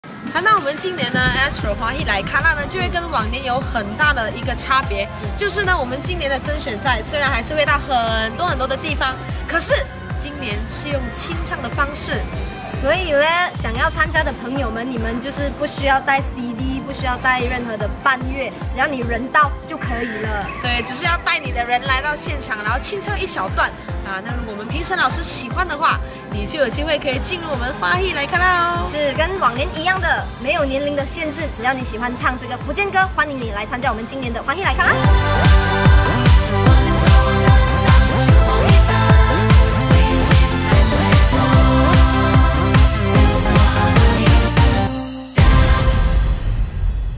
欢喜来卡拉2014 清唱试音活动
今年的《欢喜来卡拉2014 》试音活动以清唱方式进行，不用携带伴唱光碟哦～